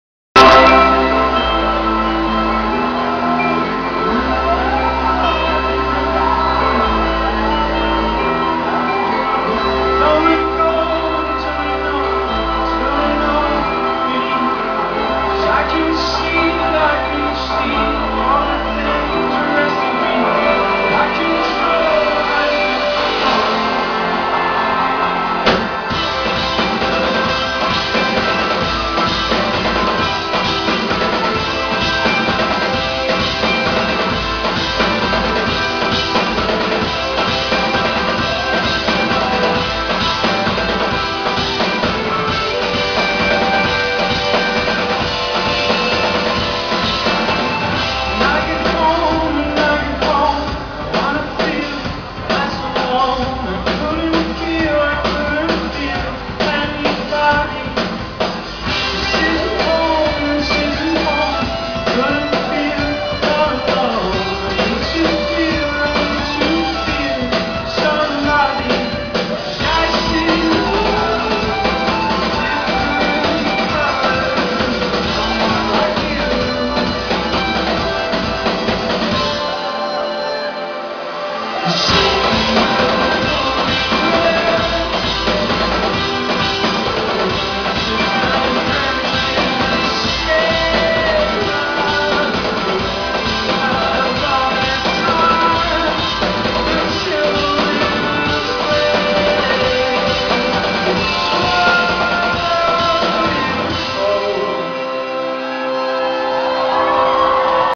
Two Door Cinema Club in Prague!
Their first concert in Prague, is so amazing to like themselves! ♥